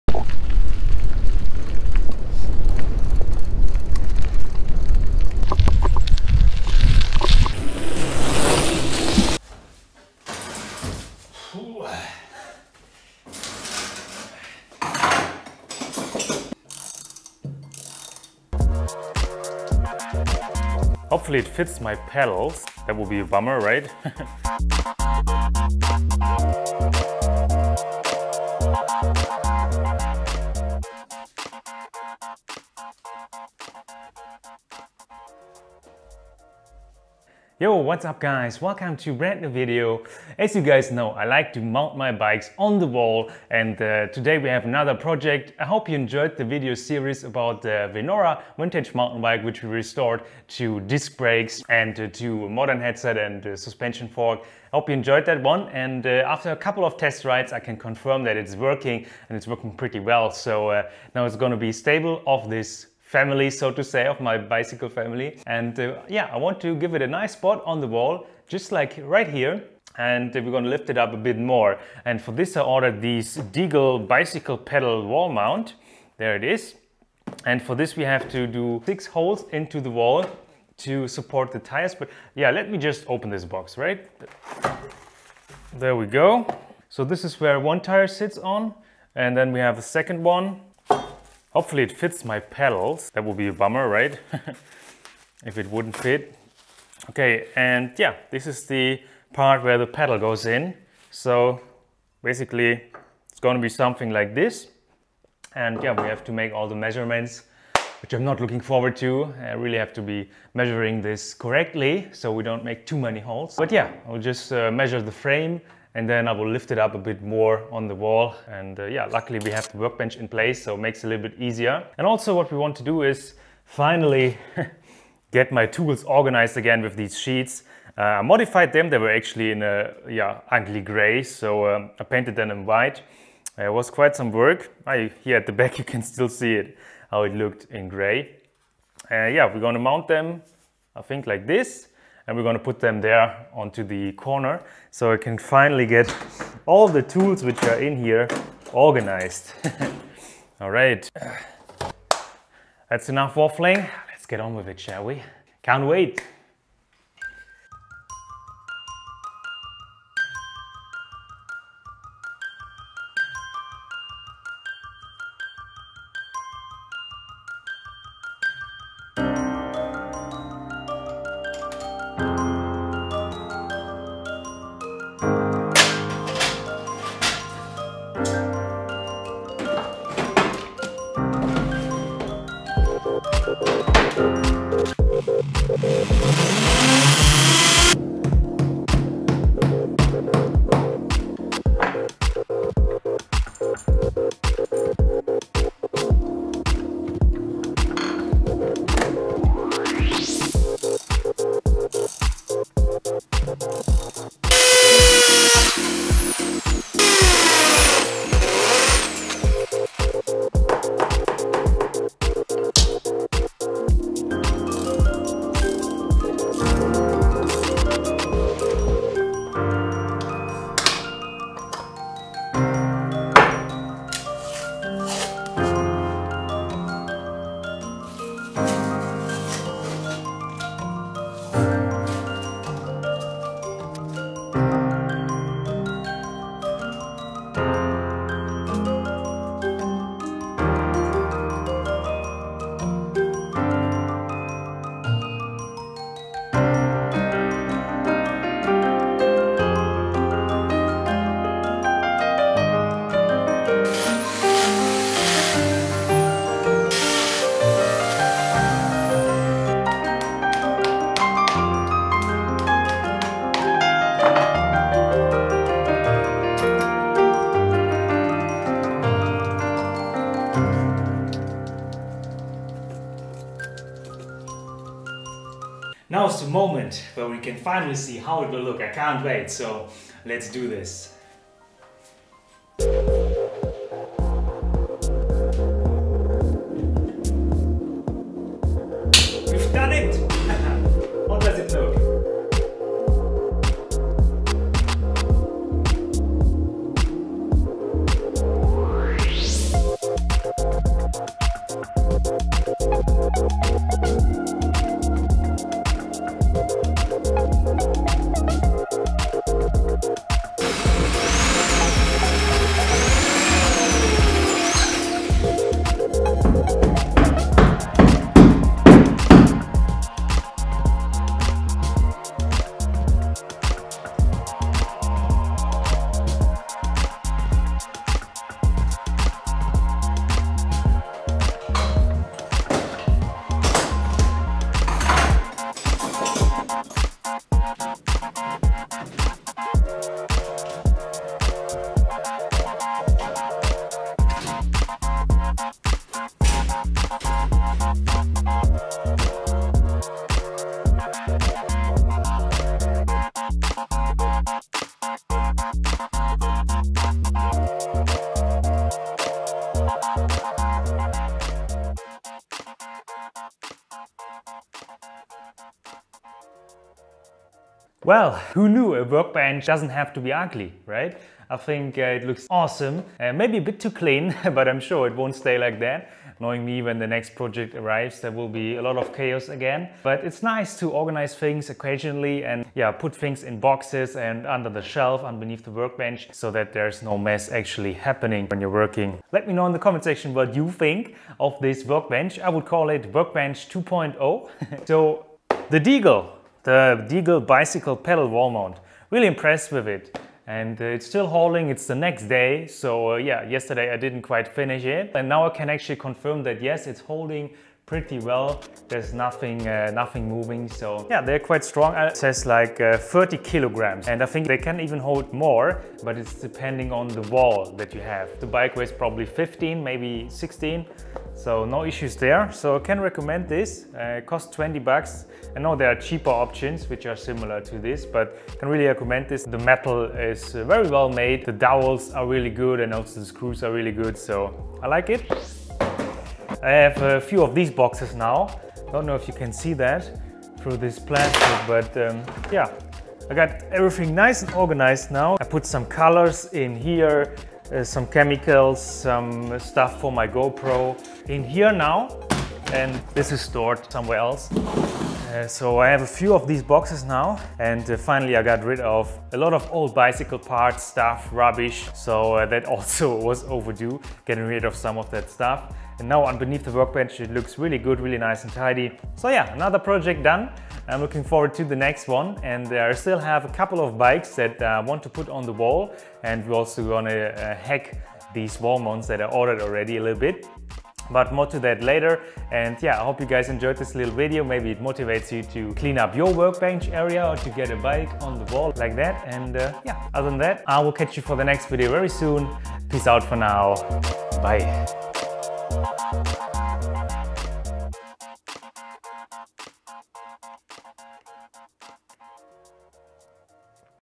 Hang your Mountainbike like this! + Workbench Project
You are looking for a Bicycle wall mount to hang up your heavy Mountainbike and make some space? Well, in this video I will show you how to hang your bike, horizantally, which does not brake the bank at all. Let´s see how it holds up! The wall mount I used here is the "Deagle Bicycle Pedal Wall Mount".